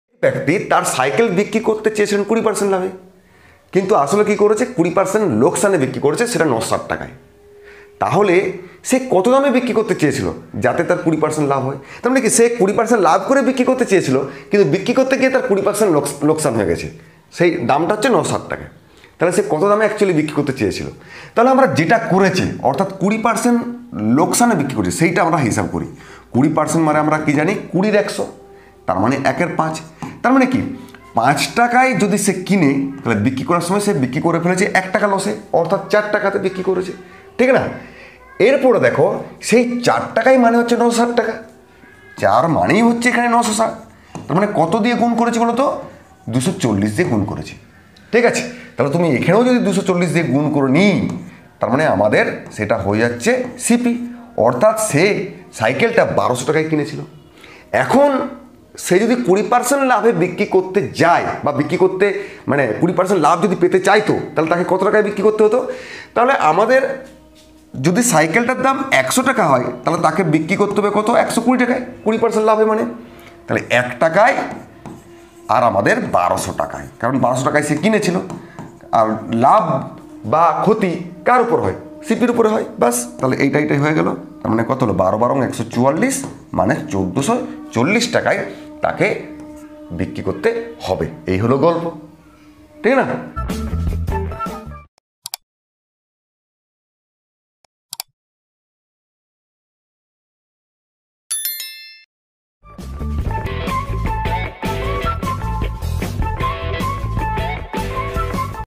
Profit and Loss Tricks | NTPC, PSC Clerkship, SSC MTS | লাভ ক্ষতির ট্রিকস মুখে মুখে | Math Tricks ❤️
Profit and Loss Tricks | NTPC, PSC Clerkship, SSC MTS | লাভ ক্ষতির ট্রিকস মুখে মুখে | Math Tricks ❤️ by @Superiormathematics

এক ব্যক্তি তার সাইকেল বিক্রি করতে চেয়েছিলেন 20% লাভে, কিন্তু আসলে 20% লোকসানে বিক্রি করে 960 টাকায়। সে কোন দামে বিক্রি করতে চেয়েছিল যাতে 20% মুনাফা অর্জন হয়?
(a) 1,420
(b) 1,440
(d) 1,340